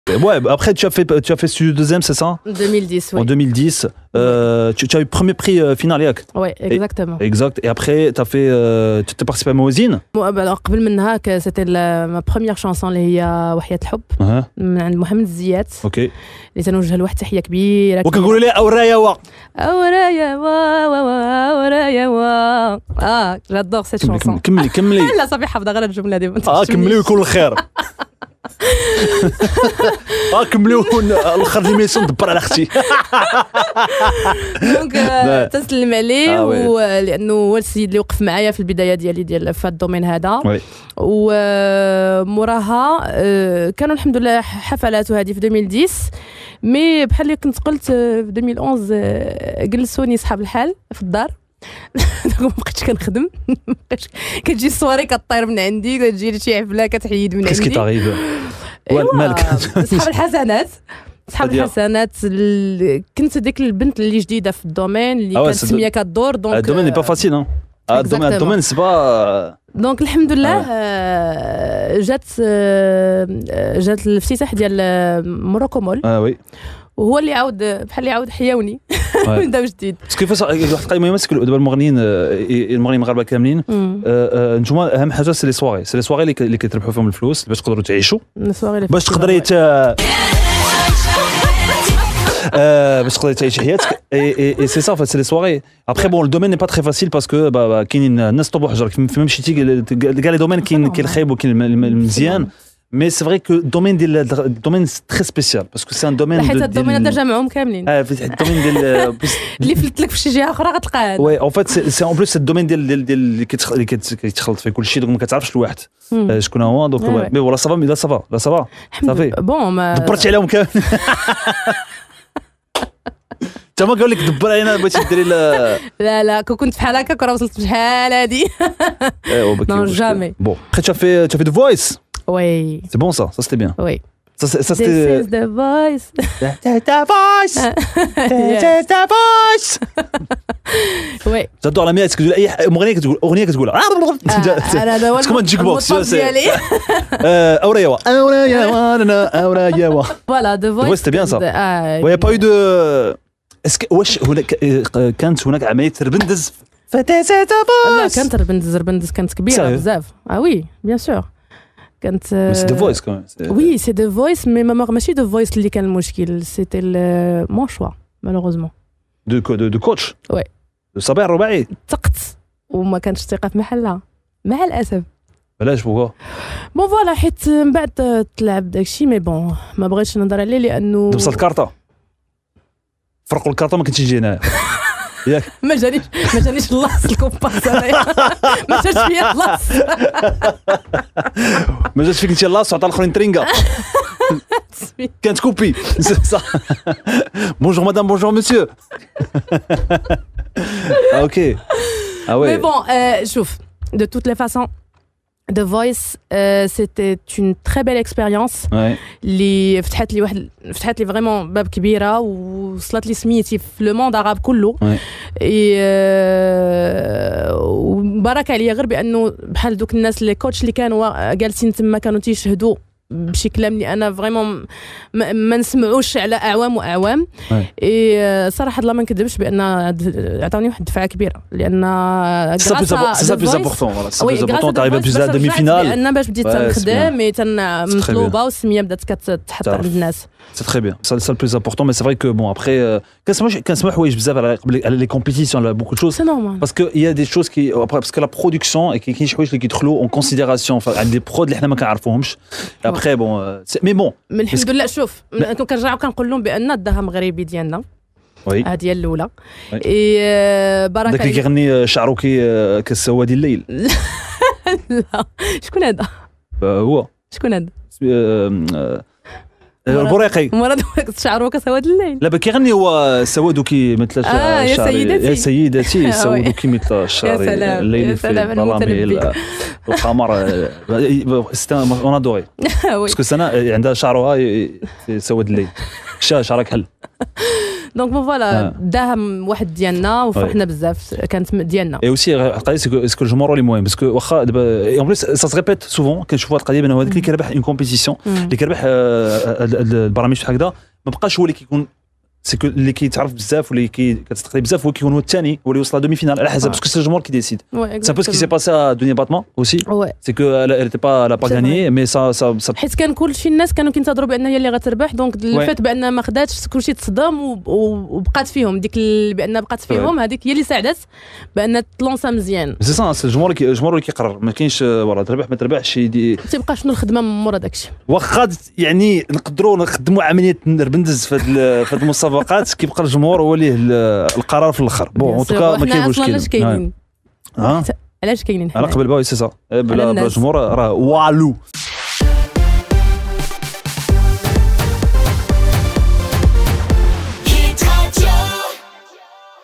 Lamia Zaydi RBANDAZ THE VOICE لمياء الزايدي عملية الربندز في
Lamia Zaydi en interview avec Momo sur Hit radio, révélations concernant le concours The Voice.